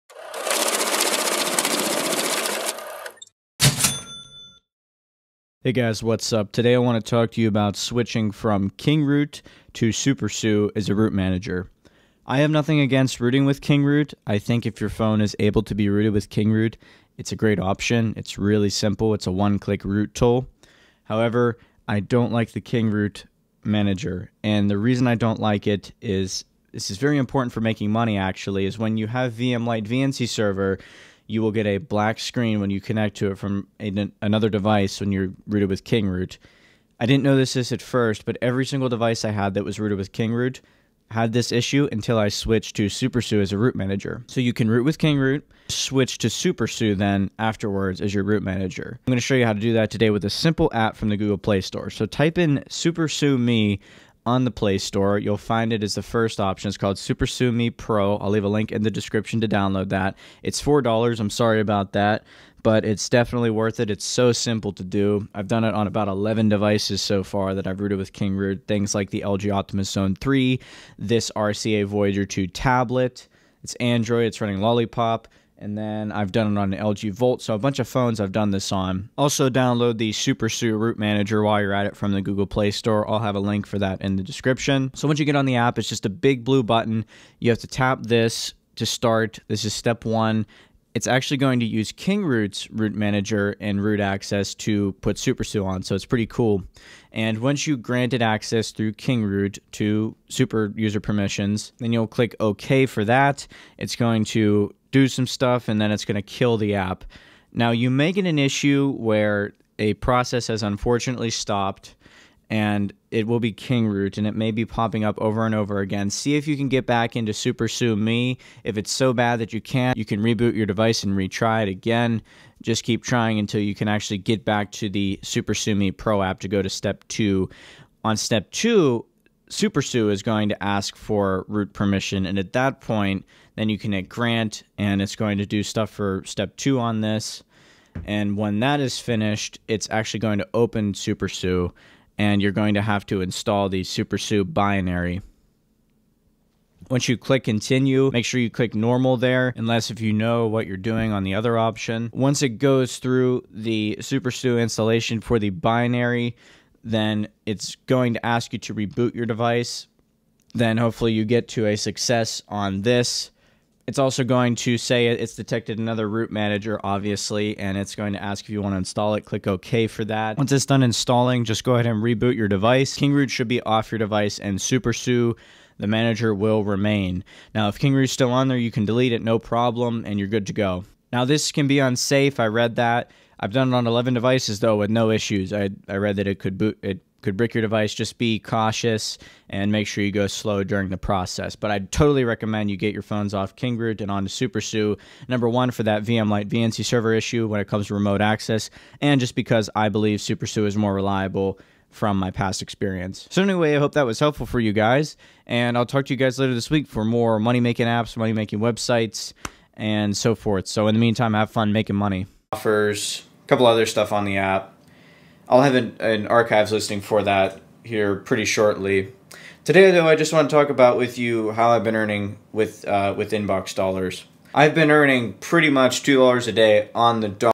Switching from KingRoot to SuperSU Android Root (Get Rid of Black Screens on VMLite VNC Server)
Come join our communities!